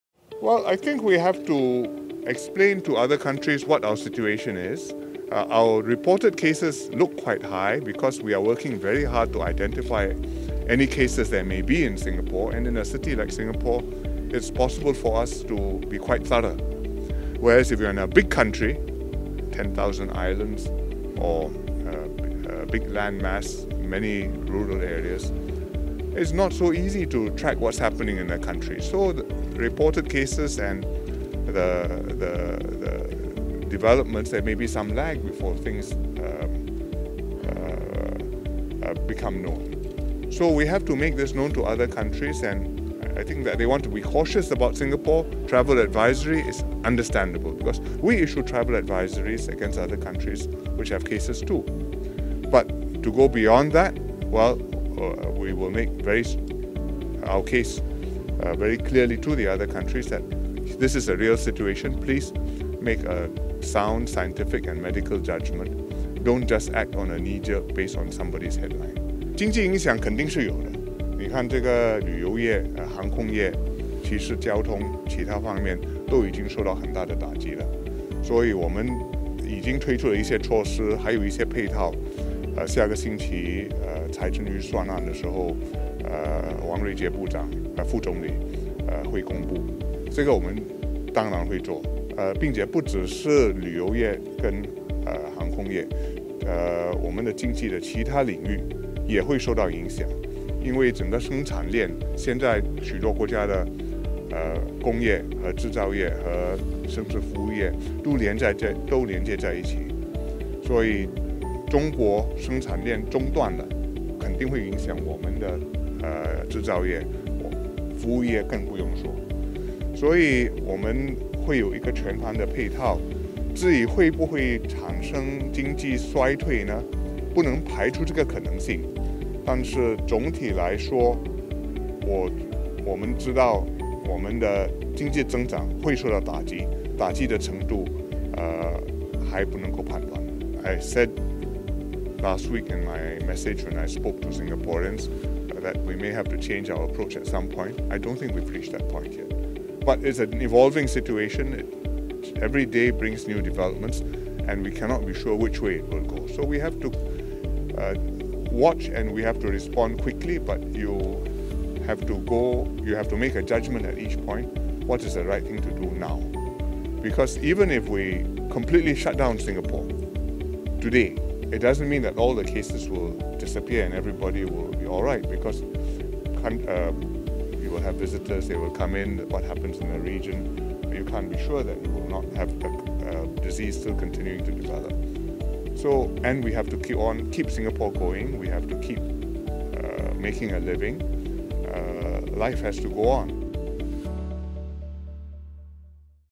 李显龙总理：受疫情影响 不排除经济陷入衰退
相较2003年沙斯时期，此次2019冠状病毒疾病（COVID-19）疫情带来的经济影响将更为显著。李显龙总理指出，不排除我国经济陷入衰退的可能性，政府下周将在预算案中宣布涵盖受影响各个领域的全盘援助配套。

李总理星期五（2月14日）上午特地到樟宜机场走一趟，探访坚守在前线各个岗位的机场员工，并且也向商家了解他们的生意如何受到影响。他在交流过程中尝试鼓励大家，提振员工的士气，但在一些人反馈机场人流大幅度减少，零售商店营业受到打击时，他也坦然地告诉大家，这是艰难时期，不清楚疫情会持续多久，但政府会尝试予以帮助。

李总理在接受记者访问时形容，这次冠病疫情暴发情况“非常紧张”，恐怕比沙斯时期还要严重，经济冲击更会持续好几个季度。
欢迎你加入我们的Telegram频道，点击 订阅zaobao.sg新闻频道！